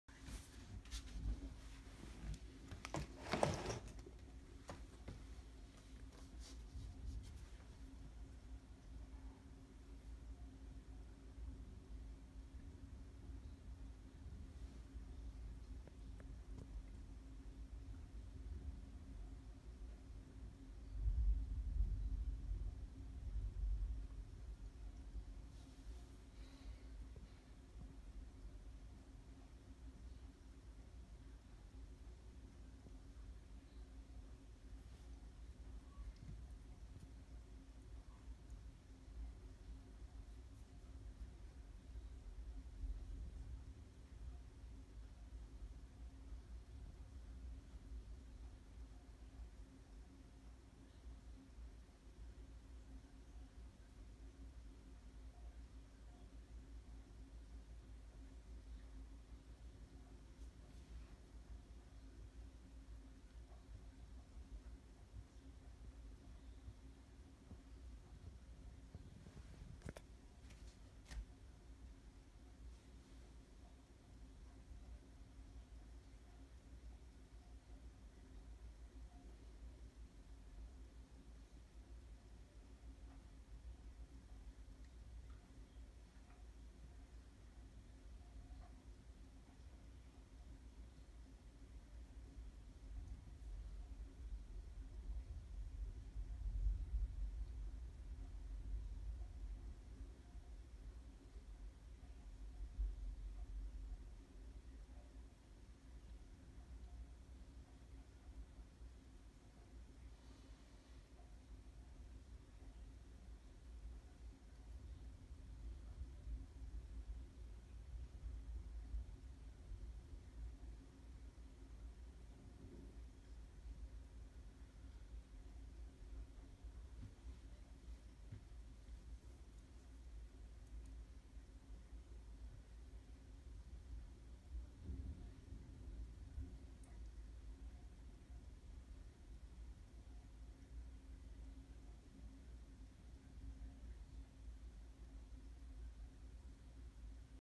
Periosteum Andouille Serve Thunderstorm Pt. 3
Thunderstorm strong continue hit residential house land same here diluted symmetrical induction in vexatious. Lots of rain continue big swift away same here obstacles dilation at repulsive.